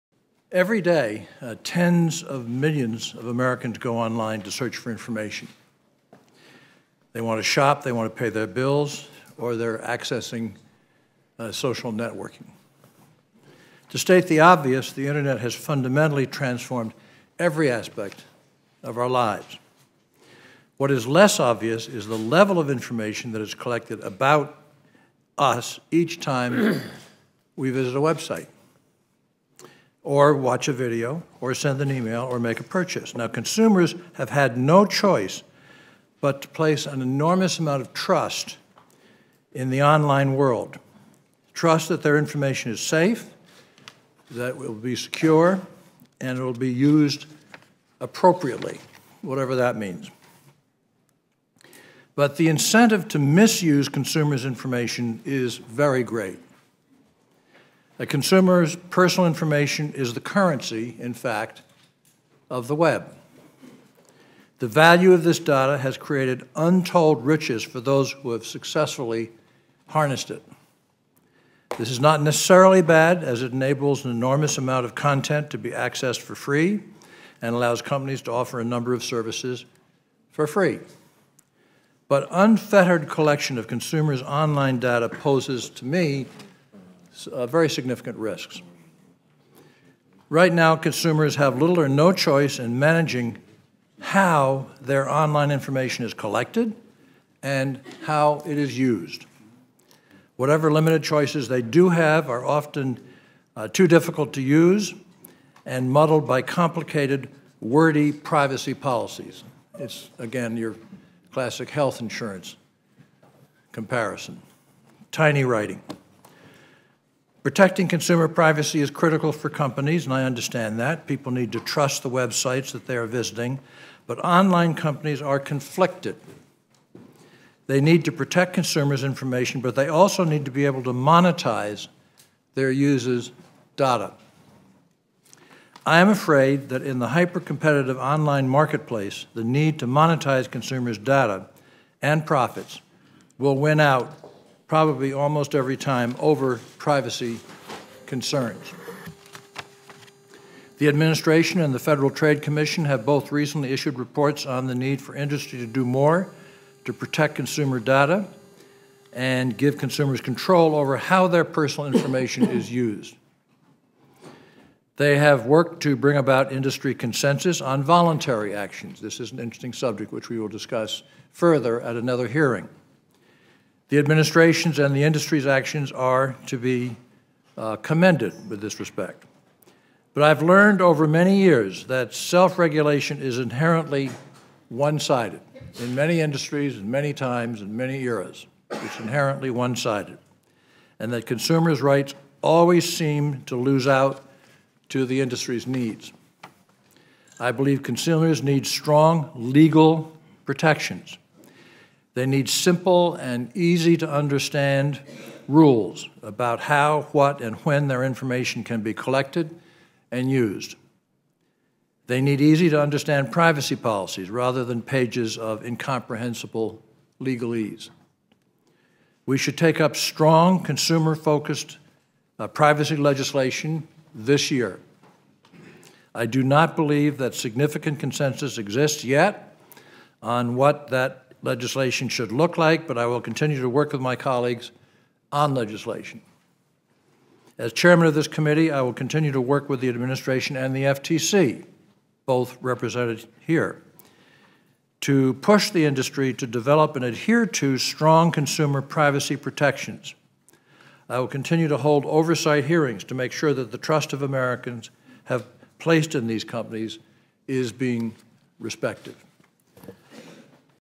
Chairman Rockefeller On Need for Privacy Protections
Chairman Rockefeller gave opening remarks at the Senate Commerce, Science, and Transportation Committee Hearing entitled "The Need for Privacy Protections: Perspectives from the Administration and the Federal Trade Commission."